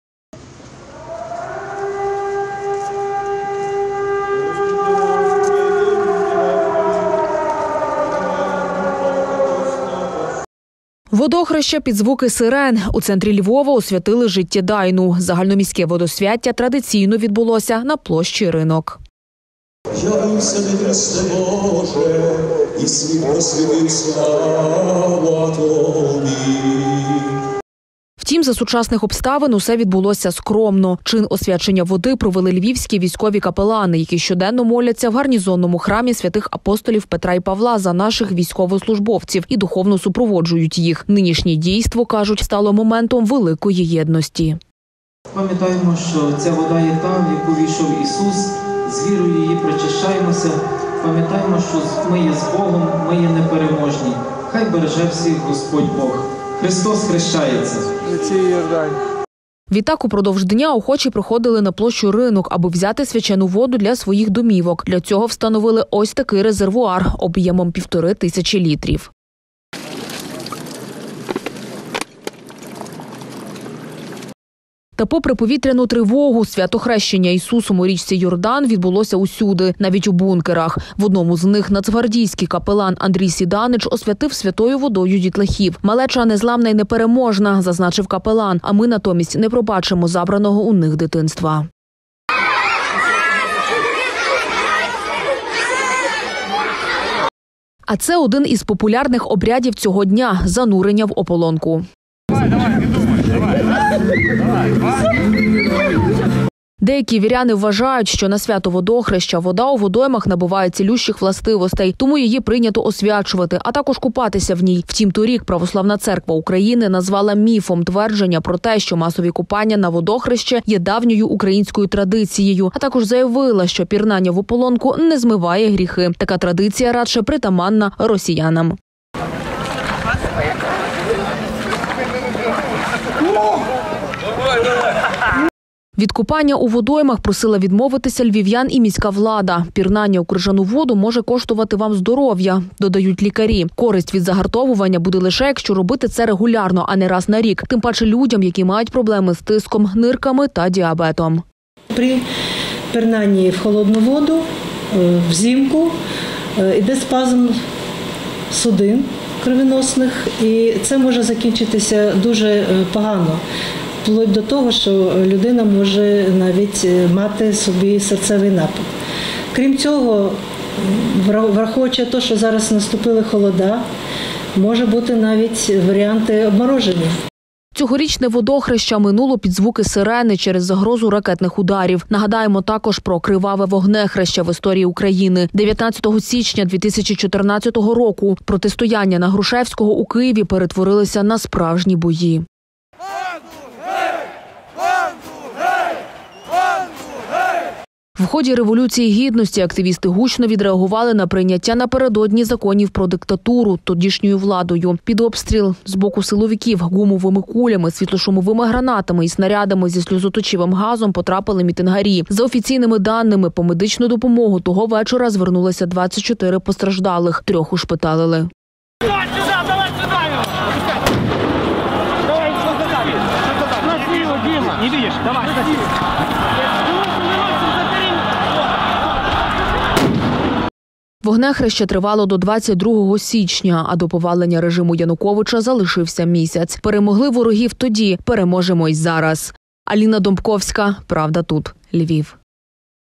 Водохреща під звуки сирен: у центрі Львова відбулося освячення води
Загальноміське водосвяття традиційно відбулося на площі Ринок. Чин освячення води провели львівські військові капелани, які щоденно моляться в Гарнізонному храмі св. апп. Петра і Павла за наших військовослужбовців і духовно супроводжують їх.